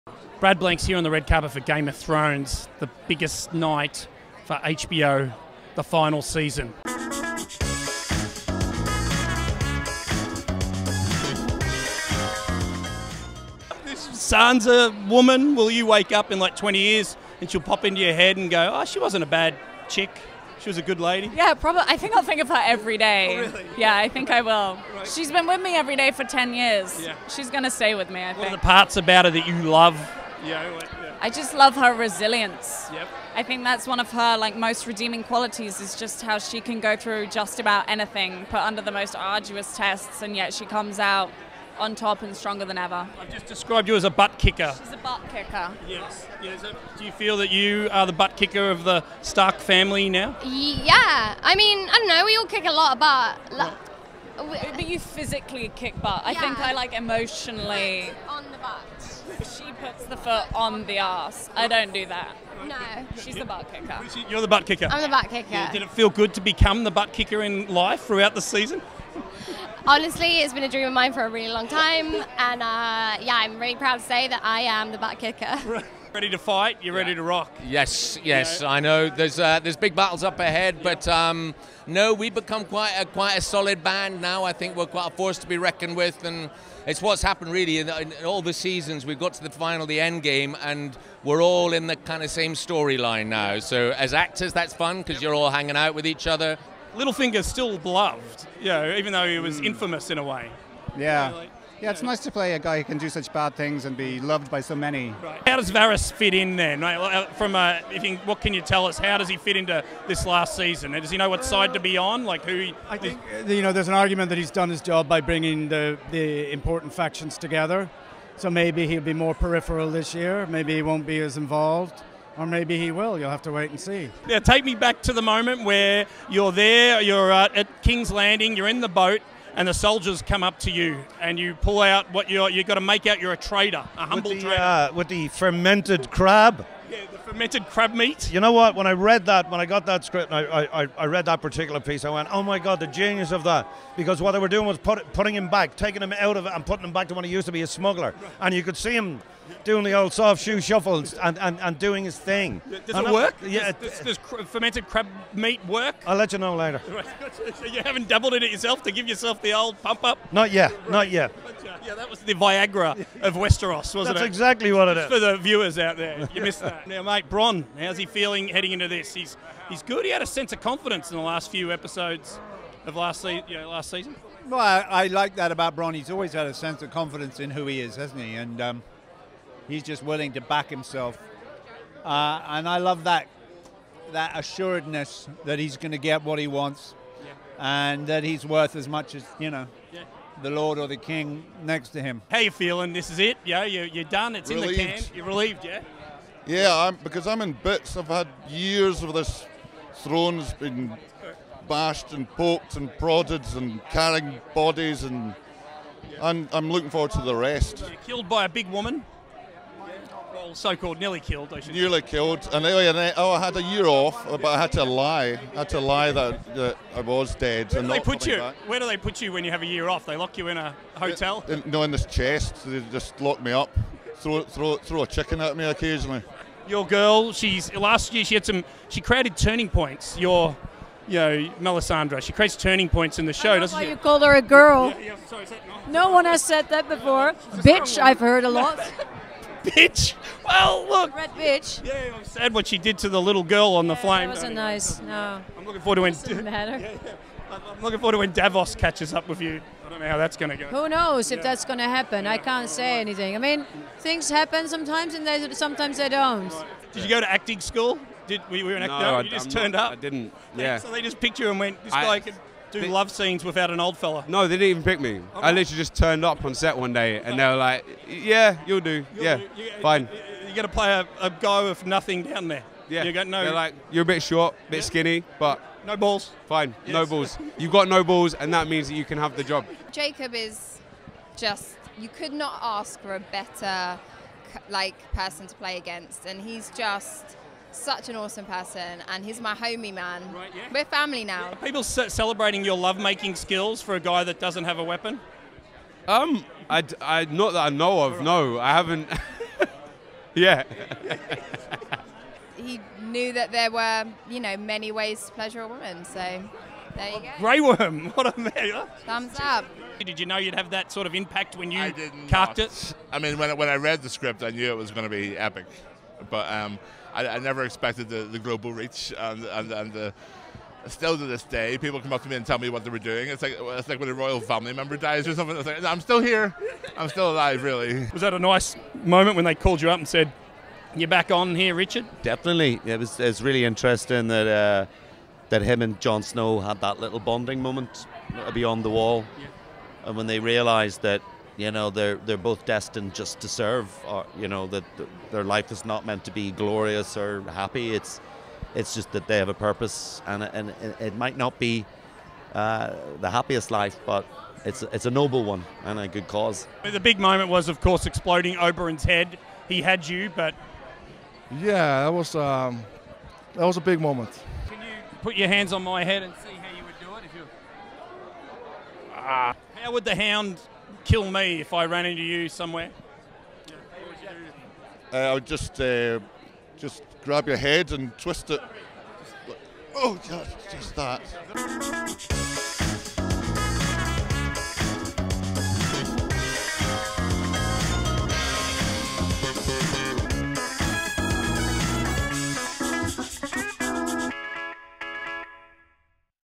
Epic GAME OF THRONES cast interviews for the final season - New York City
Brad Blanks was on the red carpet in New York City for the GAME OF THRONES red carpet. All the stars were out as they broke down the final season of this fantastic show.